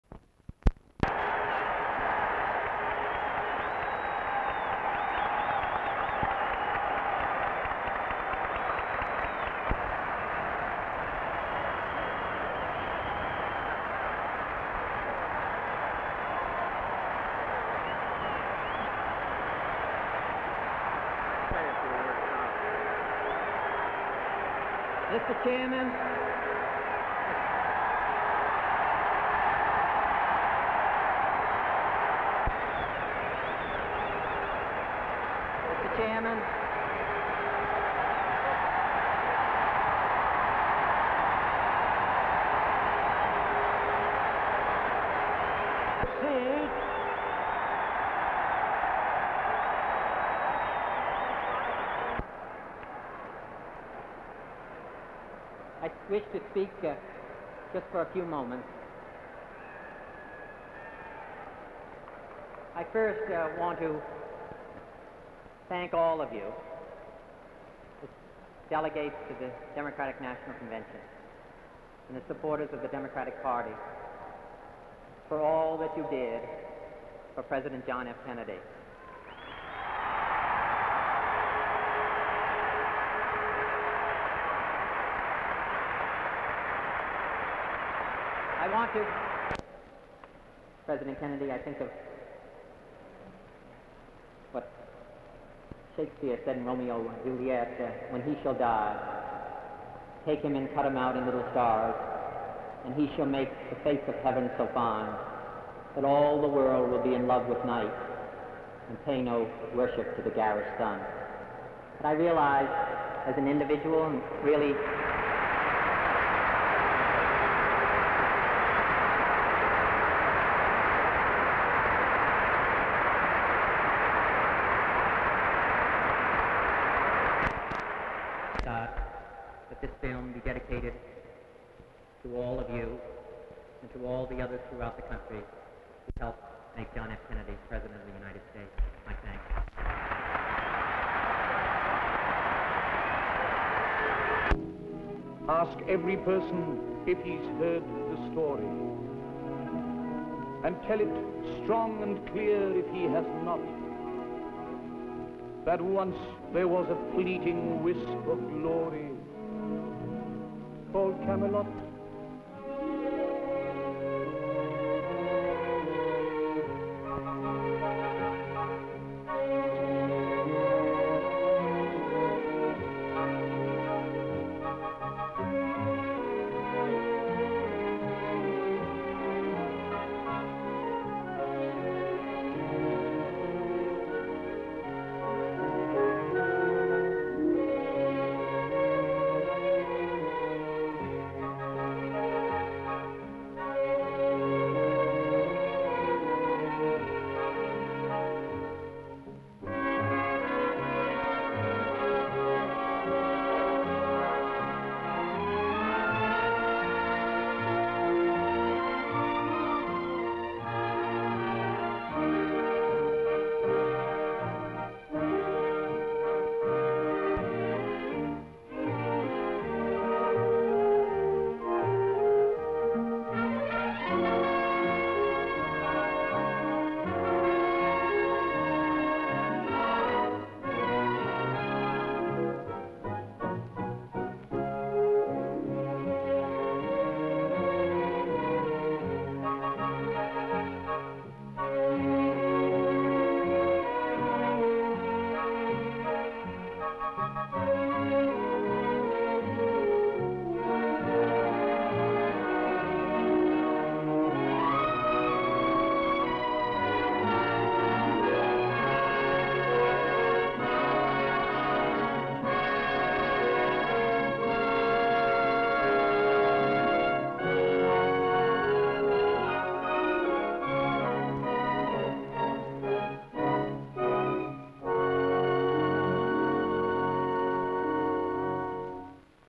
News Item (97d28147-4ea5-0882-4703-063b3f366ea0)
(20 Nov 1973) 11/20/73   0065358 - b&w
 atlantic city & others;   jfk - a man to emember.

 nxc 47657 "camelot" shows:  int convention hall,  crowd, bg sof: cu robert kennedy, bg sof:  ms rfk sof:  cu rfk sof:  ms pan crowd:  ms rfk, sof, pan to crowd:  ms rfk sof:  ms crowd:  cu rfj, sof thru walk:  ms 3 shots jfk, jackie w / kids on balcony, playing: int office jfk, kids run into office:  caroline off plane: cu john w / jackie w / little john on golf cart:  cu jfk across field:  cu jfk in boat w / others:  ms jfk w / jackie into house: mcu w / jackie, kids at beach:  mcu 2 shots jfk running on beach in water:  mcu jfk playing w / little john run to grass:  mcu jfk in boat w / little john:  ms caroline little john run to greet dad at airport: ls, bs jfk, little john walking on beach:
 (shot file film 175ft)

 kennedy, robert - sof
 kennedy, john f - sof - profile
 kennedy, john john
 kennedy, caroline
 kennedy, jackie

 xx / 175 ft / 16 b&w / print /
 opt track